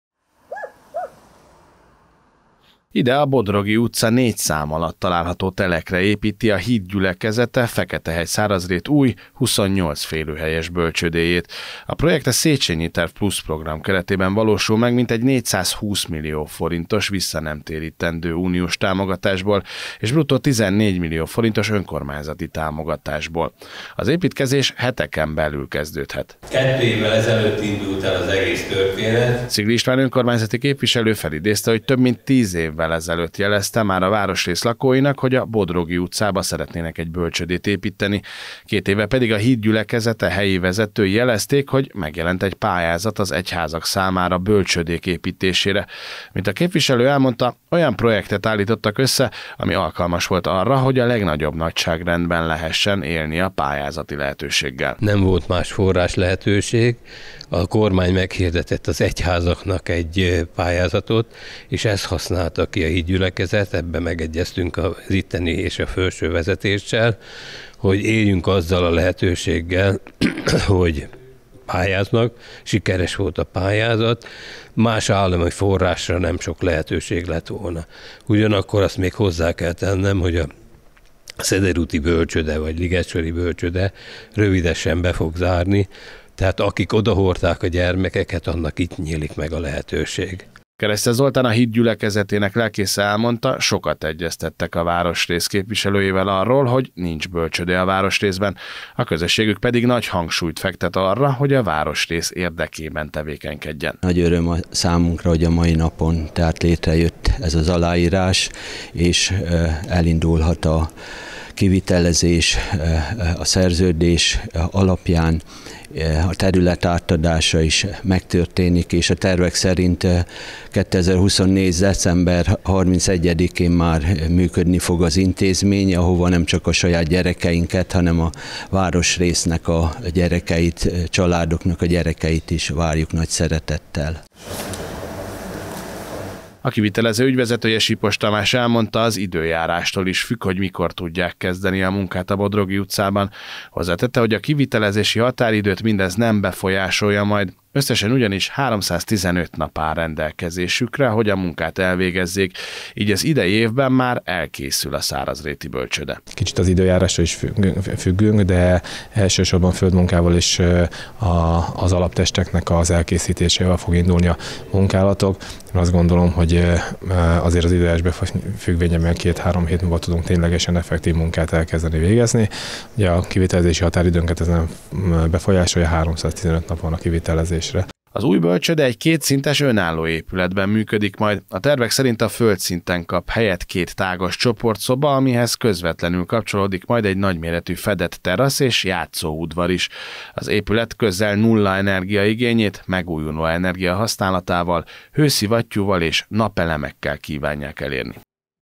ÉPÜL AZ ÚJ BÖLCSŐDE
Új, 28 férőhelyes bölcsődeépítés indul a Bodrogi utcában Feketehegy-Szárazrét gyermekei számára. A projekt a Széchenyi Terv Plusz program keretében valósul meg 420 millió forint vissza nem térítendő európai uniós támogatás segítségével. A Hit Gyülekezete fenntartásában működő intézmény várhatóan 2024. év végén fogadja majd a beiratkozókat.